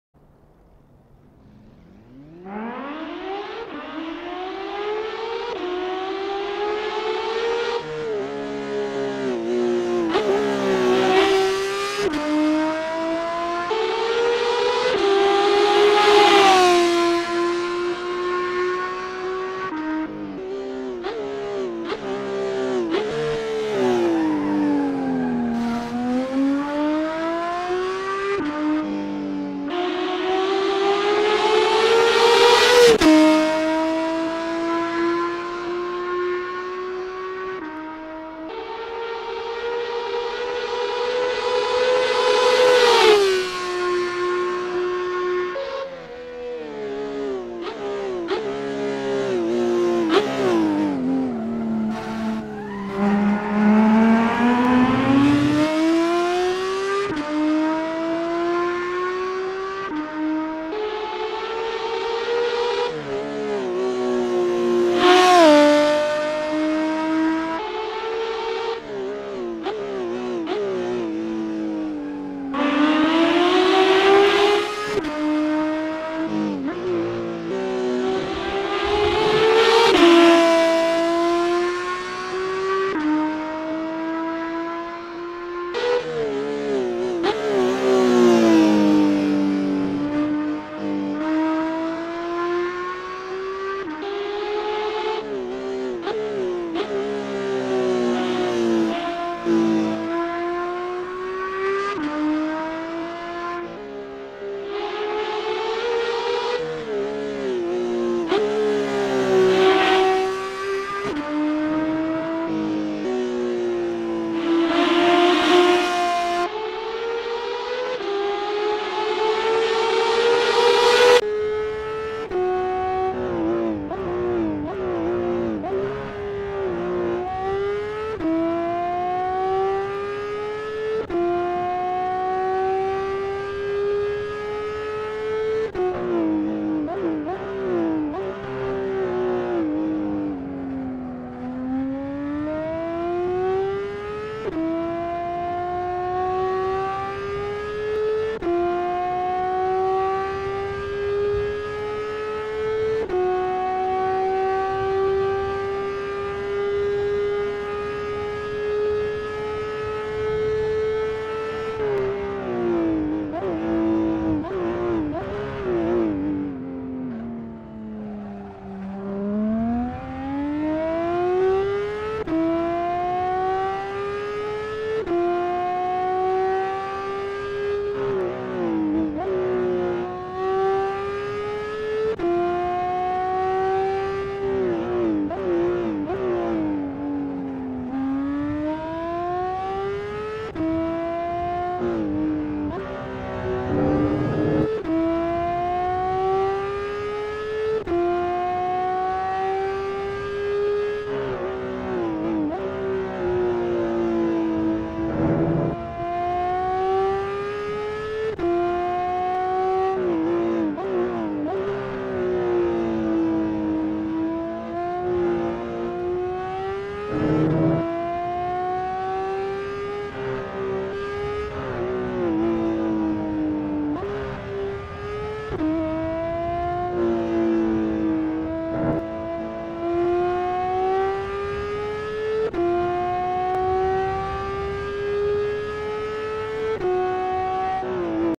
Porsche 911 GT3 RS Weissach Package Soundmod @ Hockenheimring GarryReal + Reshade
Computer Specs:
AMD Ryzen 7 5800x3d
AMD RX7800XT 16Gb
64Gb of DDR4 ram

Porsche 911 GT3 RS Weissach Package with Soundmod (join for free)